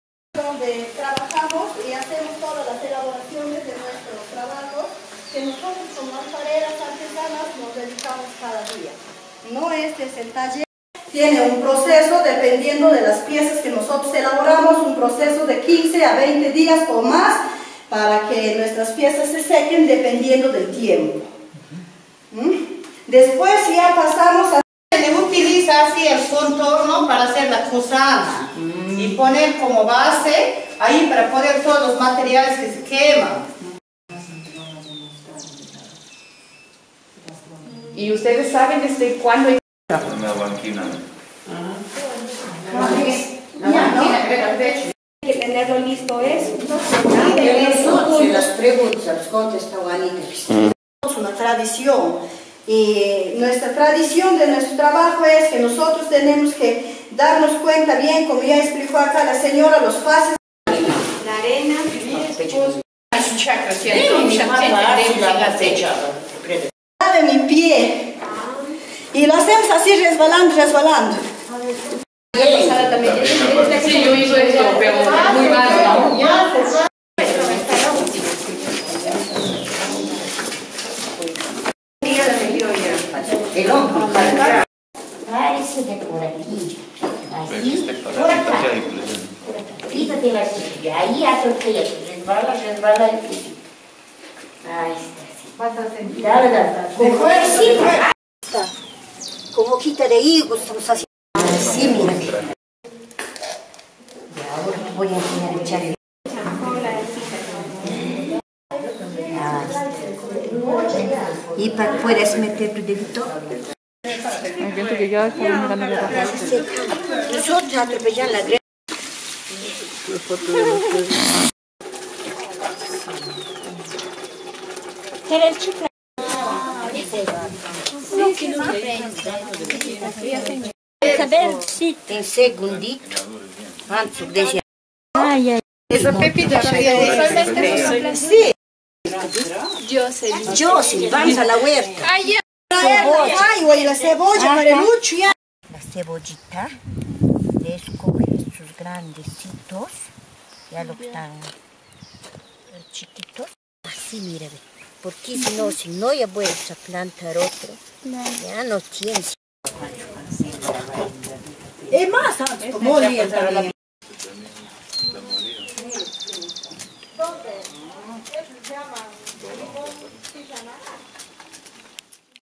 Activité céramique à Huancas
Huancas est un village à 15 minutes de Chachapoyas. Vous pouvez y aller en colectivo pour visiter les 2 miradores du Canyon del Sonche de Huanca Urcu et Sonche.

Si vous le souhaitez, vous pouvez aussi participer à une activité de céramique, une tradition millénaire des femmes de Huancas qui se transmet toujours de mère en fille. Vous verrez que ce n'est pas du tout repos, mais un travail physique !